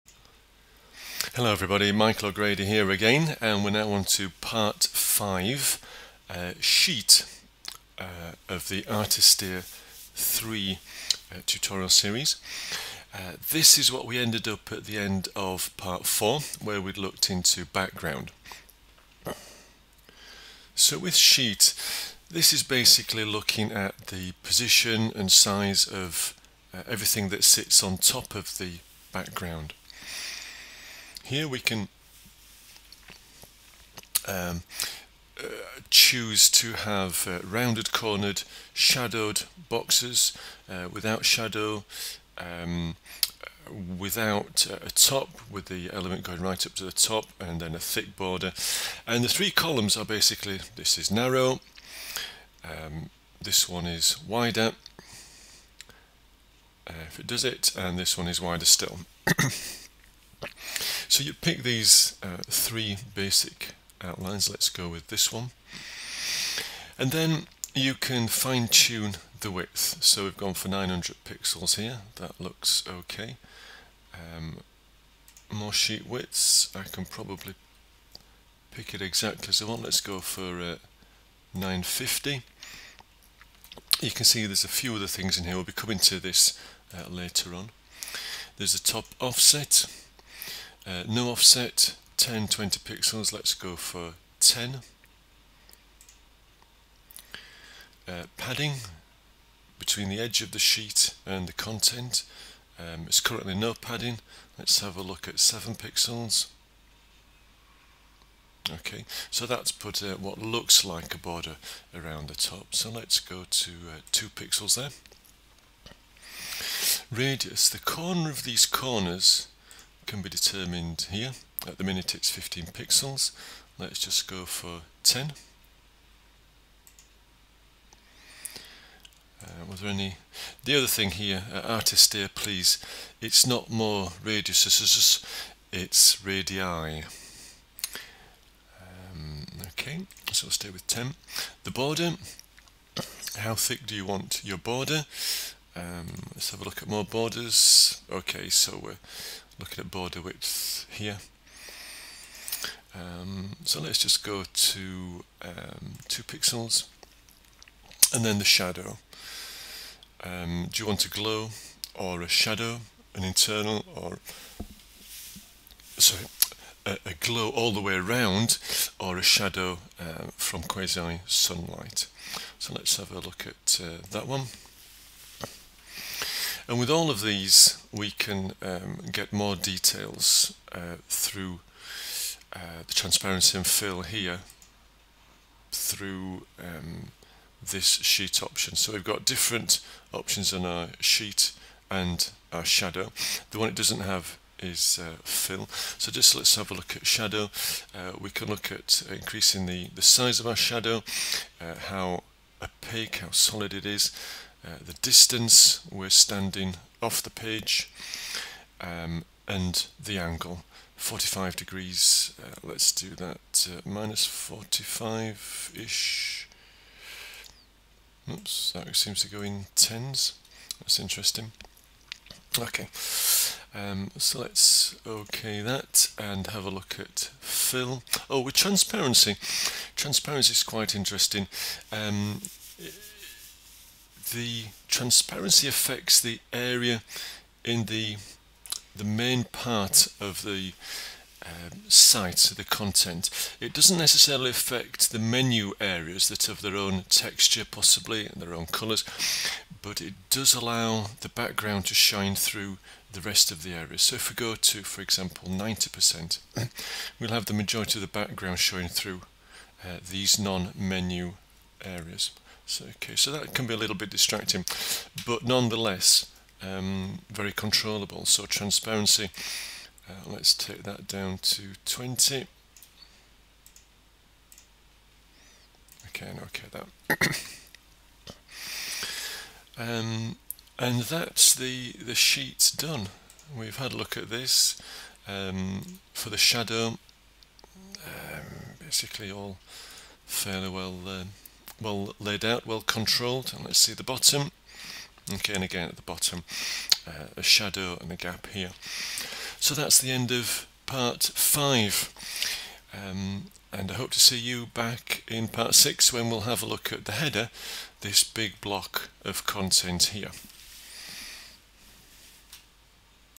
Artisteer Tutorial WordPress 5/14 Sheet
Part 5 of 14 - Artisteer 3: Sheet

Artisteer 3 is surely the ultimate template maker for WordPress, Drupal, Joomla, DotNetNuke, Blogger etc. When you make a WordPress blog, the standard free themes are reasonable and there is quite a selection.

But why opt for second best when you can make your own with Artisteer 3. Did you see Part 4 yet - Background

This is Part 5 and looks at the Sheet tab. Be sure to watch the preceding parts.

Michael   :-)